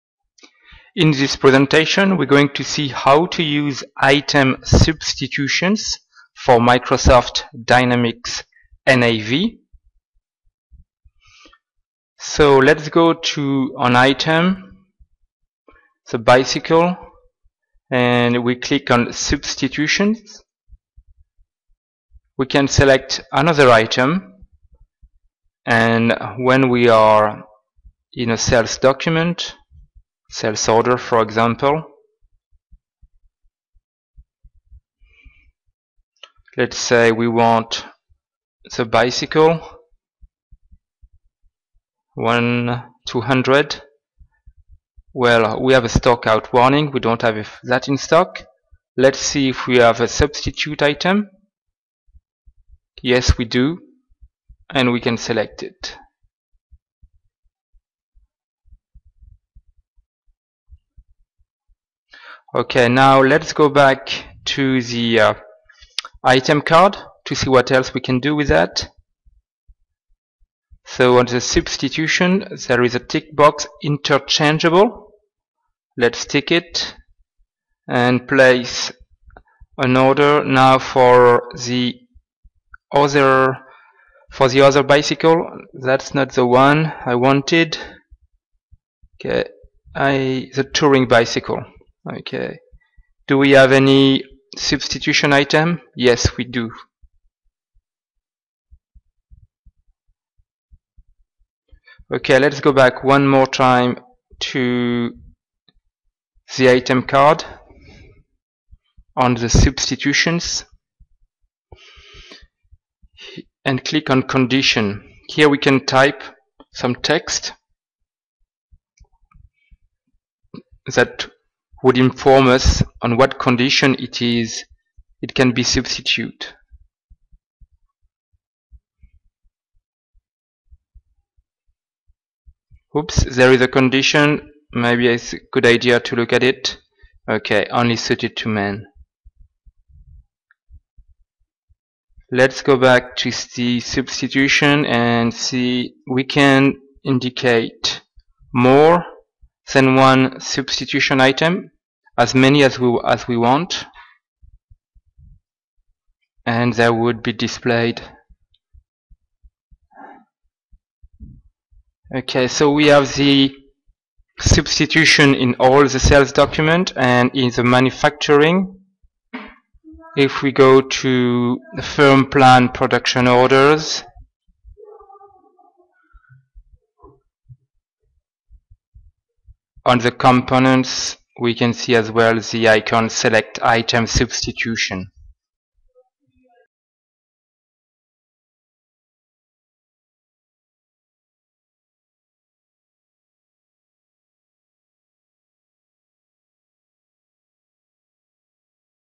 How to use Dynamics NAV Item Substitutions
How to use Dynamics NAV Item Substitutions, presented by Match Business Solutions